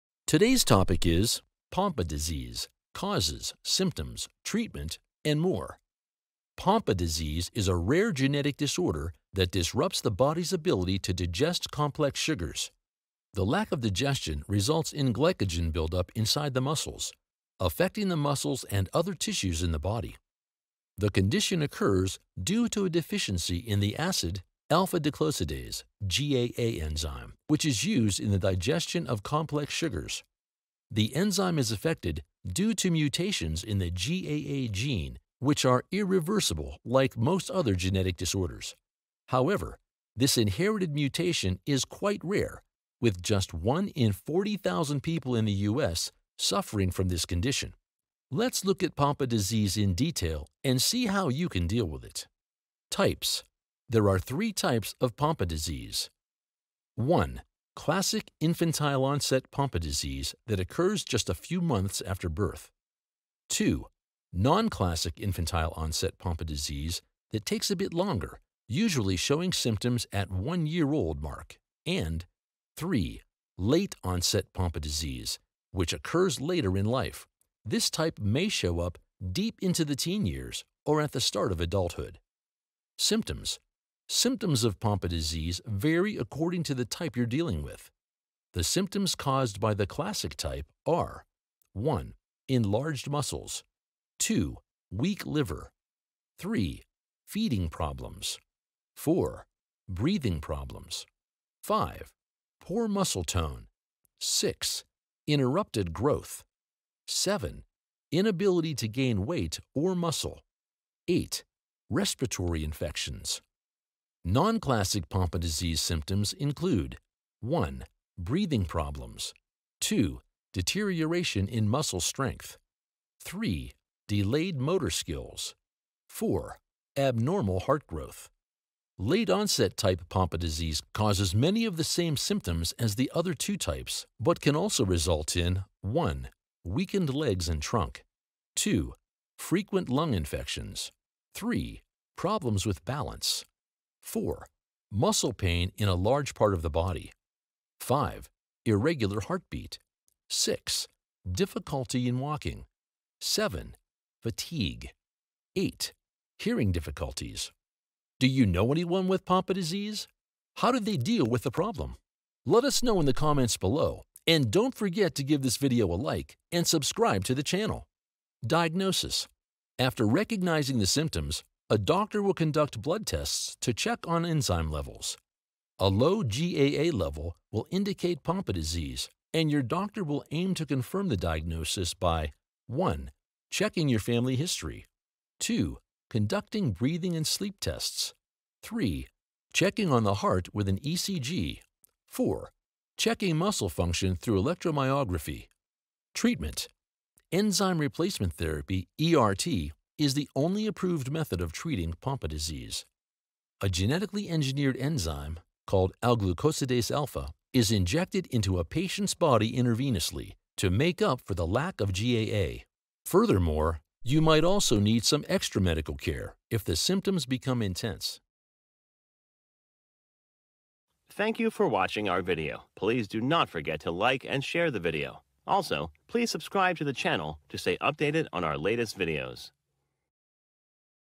Pompe Disease: Causes, Symptoms, Treatment and More
Chapters

0:00 Introduction
0:53 Types of Pompe Disease
1:23 Symptoms of Pompe Disease
2:48 Diagnosis for Pompe Disease
3:16 Treatment for Pompe Disease

● Pompe disease is a rare genetic disorder that disrupts the body’s ability to digest complex sugars.
● The lack of digestion results in glycogen buildup inside the muscles, affecting the muscles and other tissues of the body.
● The condition occurs due to a deficiency of the acid alfa glucosidase (GAA) enzyme, which is used in the digestion of complex sugars.
● The enzyme is affected due to mutations in the GAA gene, which are irreversible like most other genetic disorders.
● However, this inherited mutation is quite rare, with just 1 in 40,000 people in the US suffering from this condition.
● Let’s look at Pompe disease in detail and see how you can deal with it:
Types
● There are three types of Pompe disease:
1. Classic infantile-onset Pompe disease that occurs just a few months after birth.
2. Non-classic infantile-onset Pompe disease that takes a bit longer, usually showing symptoms at the one-year-old mark, and
3. Late-onset Pompe disease, which occurs later in life. This type may show up deep into the teen years or at the start of adulthood.
Symptoms
● Symptoms of Pompe disease vary according to the type you’re dealing with.
● The symptoms caused by the classic type are:
1. Enlarged muscles
2. Weak liver
3. Feeding problems
4. Breathing difficulties
5. Poor muscle tone
6. Interrupted growth
7. Inability to gain weight or muscle
8. Respiratory infections
● Non-classic Pompe disease symptoms include:
1. Breathing problems
2. Deterioration in muscle strength
3. Delayed motor skills
4. Abnormal heart growth
● Late-onset type Pompe disease causes many of the same symptoms as the other two types, but can also result in:
1. Weakened legs and trunk
2. Frequent lung infections
3. Problems with balance
4. Muscle pain in a large part of the body
5. Irregular heartbeat
6. Difficulty in walking
7. Fatigue
8. Hearing difficulties
Do you know anyone with Pompe disease? How did they deal with the problem?
Diagnosis
● After recognizing the symptoms, a doctor will conduct blood tests to check on enzyme levels.
● A low GAA level will indicate Pompe disease, and your doctor will aim to confirm that diagnosis by:
1. Checking your family history
2. Conducting breathing and sleep tests
3. Checking on the heart with an ECG
4. Checking muscle function through an electromyography
Treatment
● Enzyme replacement therapy (ERT) is the only approved method of treating Pompe disease.
● A genetically engineered enzyme called alglucosidase alfa is injected into a patient’s body intravenously to make up for the lack of GAA.
● Furthermore, you might also need some extra medical care if the symptoms become intense.